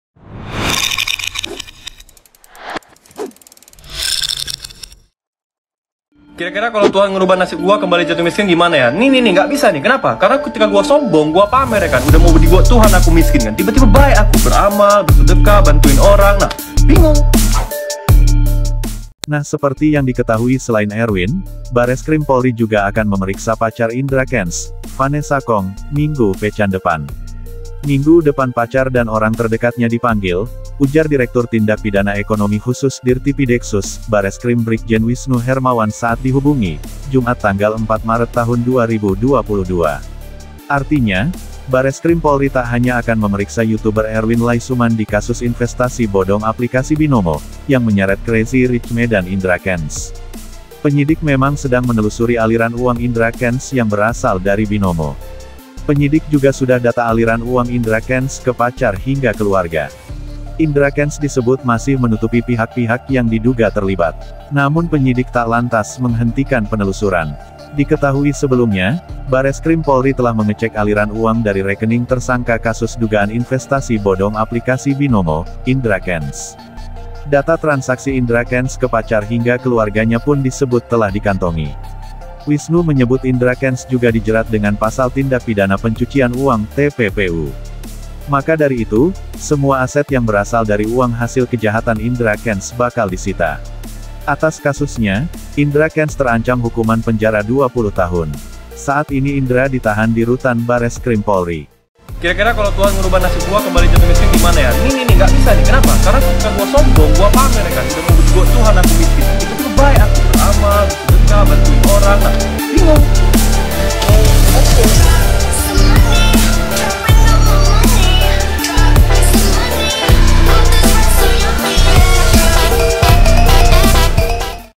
RESMI, Bareskrim Periksa Pacar Indra Kenz, Semua Aset Vanessa Khong Akan Disita!!
Nah seperti yang diketahui Selain Erwin, Bareskrim Polri juga akan memeriksa pacar Indra Kenz, Vanessa Khong, minggu pekan depan. “Minggu depan pacar dan orang terdekatnya dipanggil,” ujar Direktur Tindak Pidana Ekonomi Khusus (Dirtipideksus) Bareskrim Brigjen Whisnu Hermawan saat dihubungi, Jumat 4 maret 2022.

Selamat datang di channel Topik Viral.